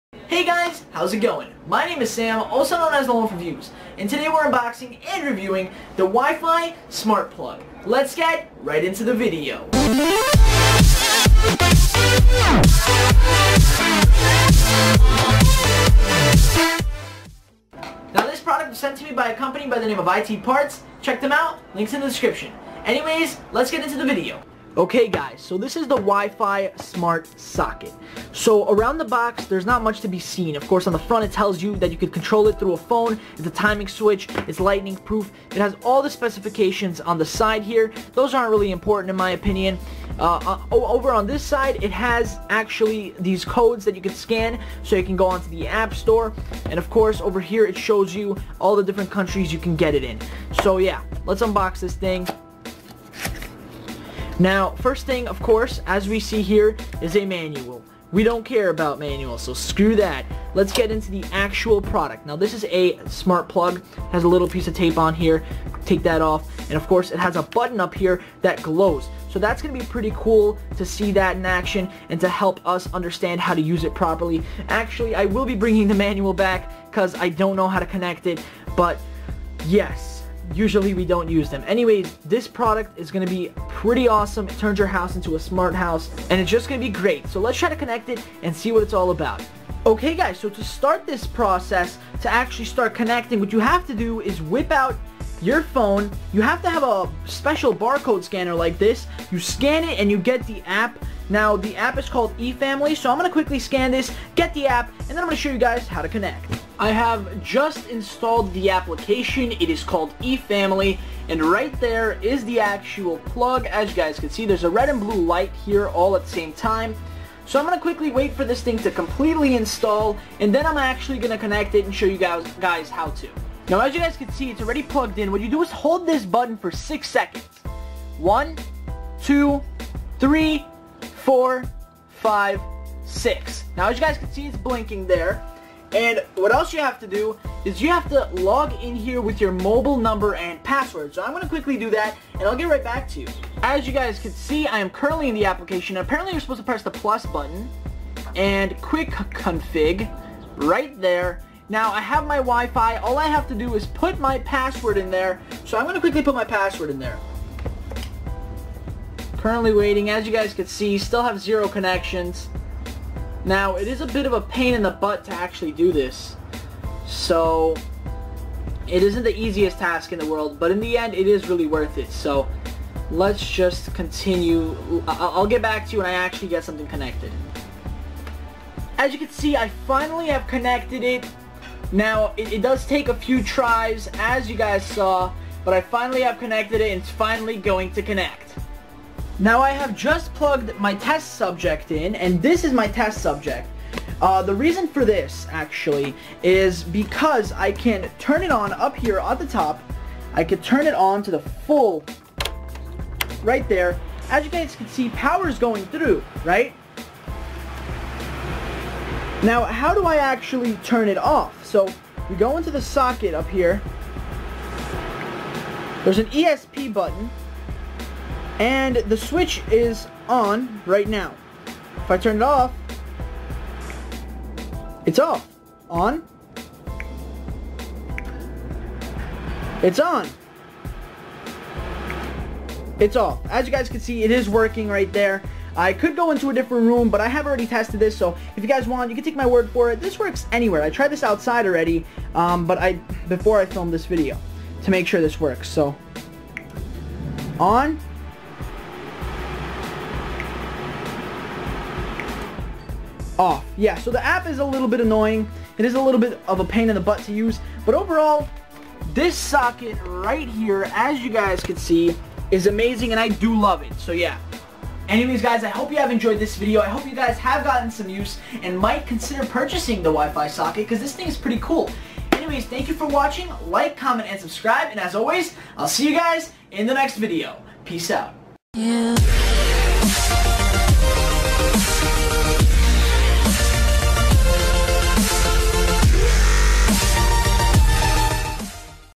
How to Control Everything With Your Phone!!!! ( Wi-Fi Smart Socket Unboxing and Review)
Today we unbox and review a smart wifi plug that will turn your ordinary house into the smartest house on the block. its easy to connect and is better than many of the competitors for price and performance.